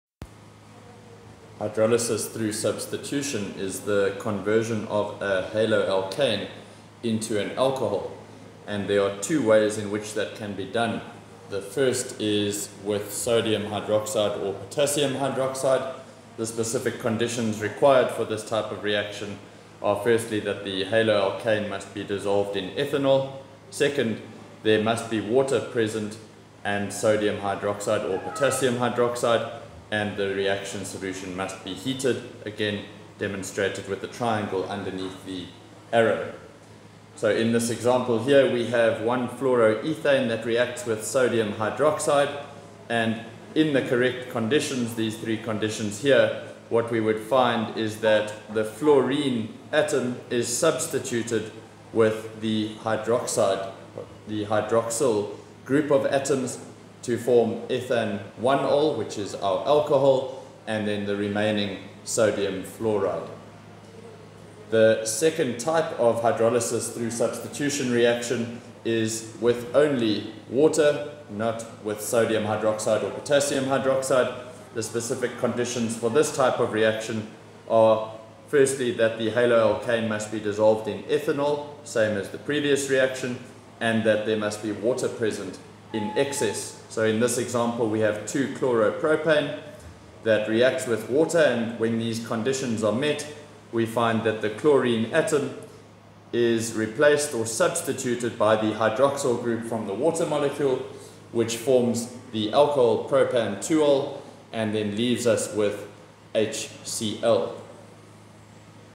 Hydrolysis through Substitution
This video explains halogenation as a substitution reaction.

South Africa
CAPS
Grade 12
Organic Chemistry